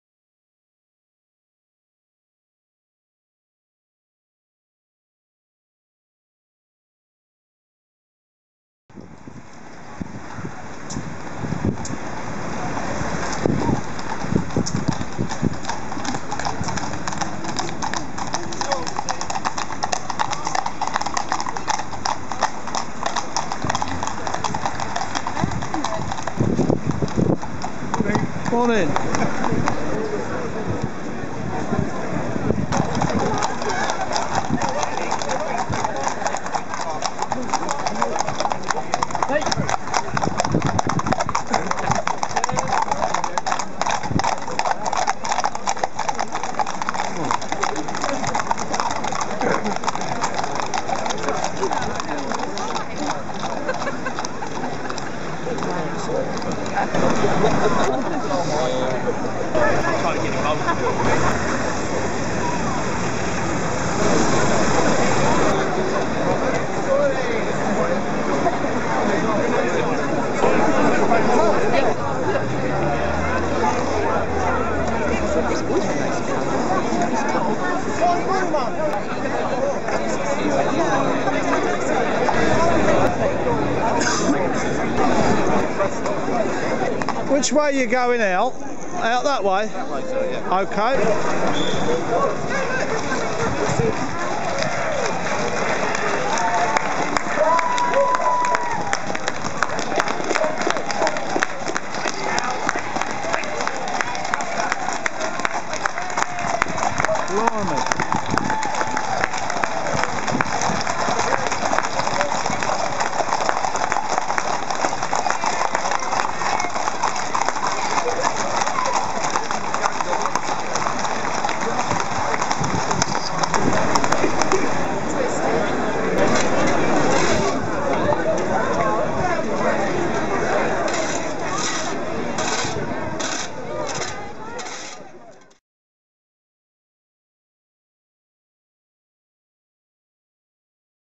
Ashford Valley Hunt 2010.
Boxing day meet at Tenterden Kent on Bank Holiday Monday 27th December at the Vine Inn.